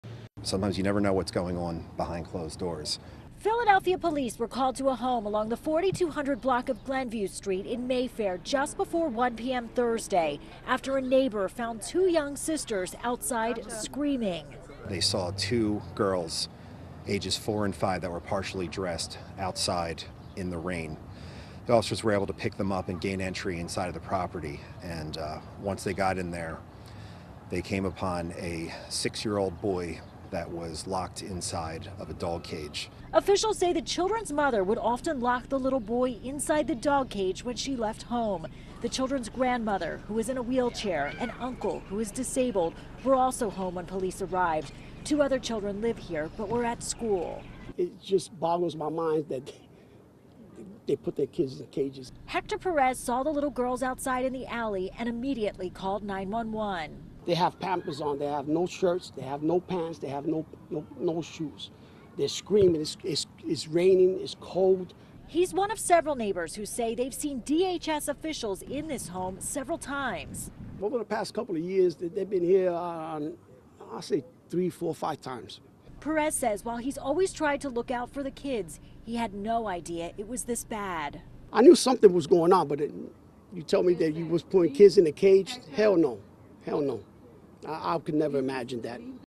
PA: 6-year-old found naked inside dog cage
Philadelphia police found a 6-year-old boy naked inside a dog cage Thursday after a neighbor called 911 when he saw two young girls outside, partially dressed and screaming.